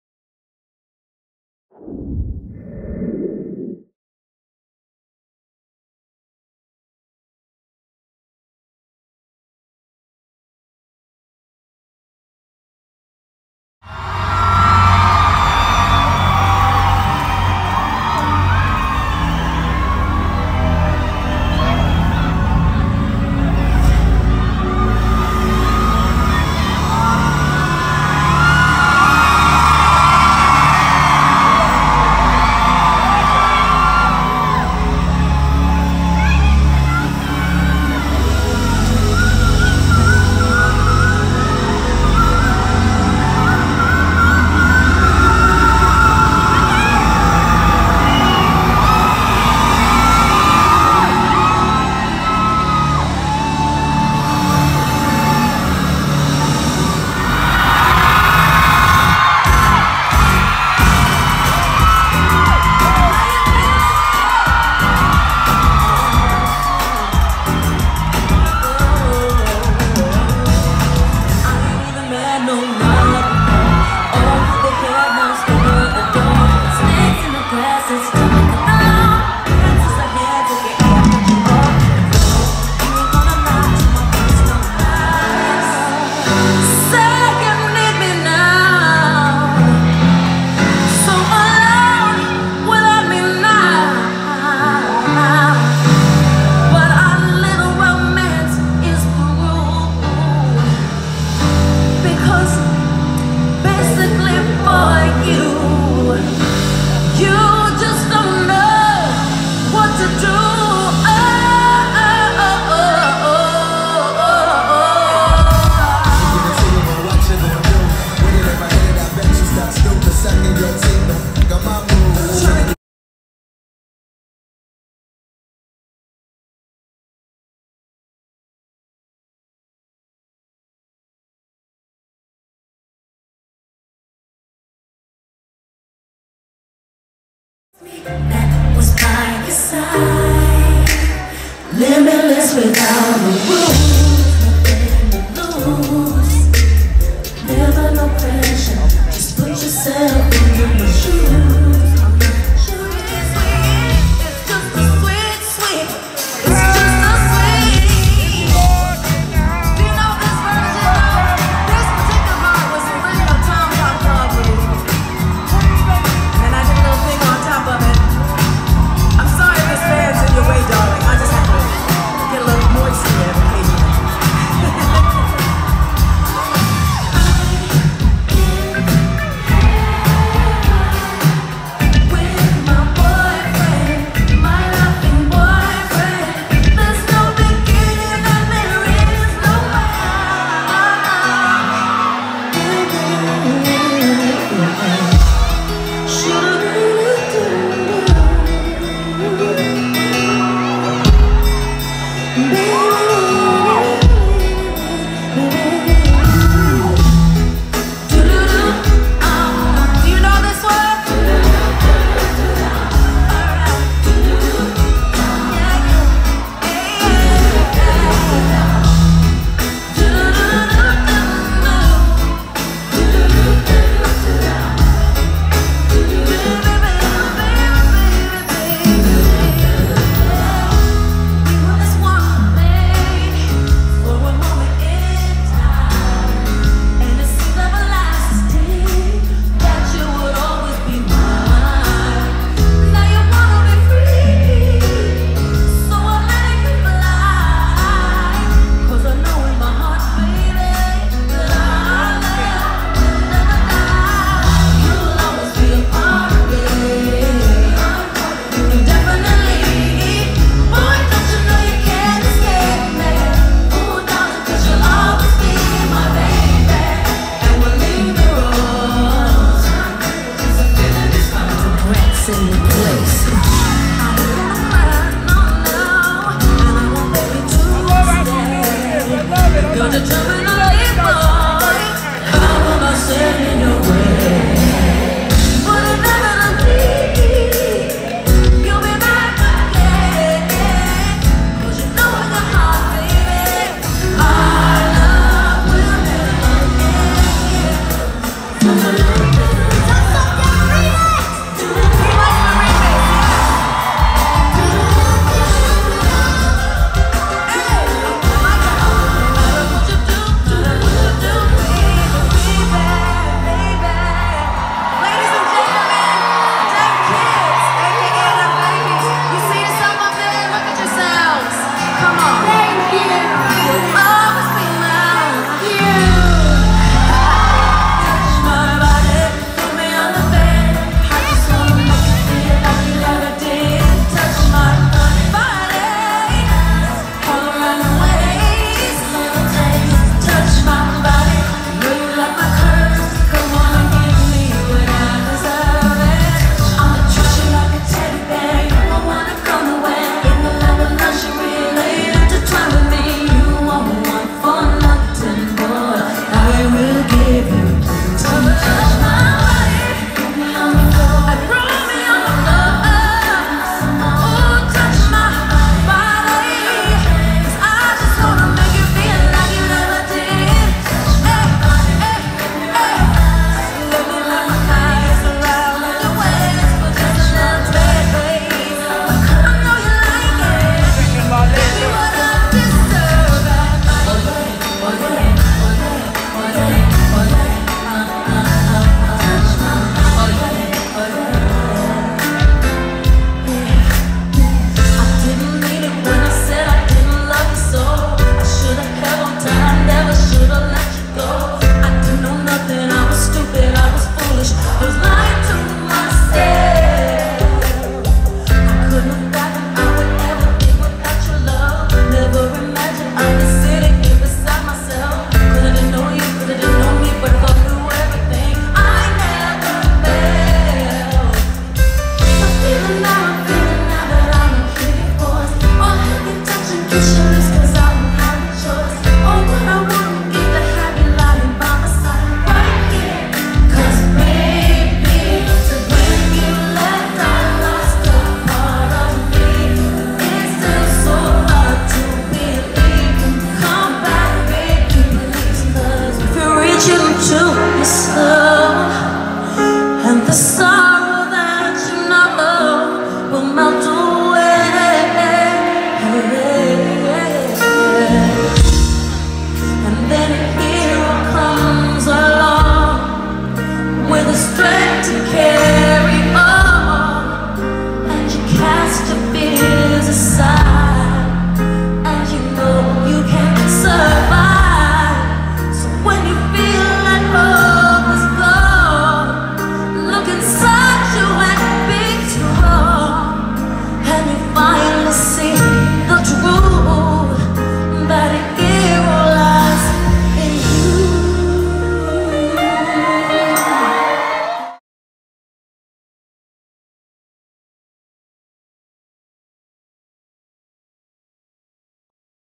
Mariah’s cautions tour.
The diva herself came to Indianapolis and performed such an intimate show. Hope you all enjoy the footage.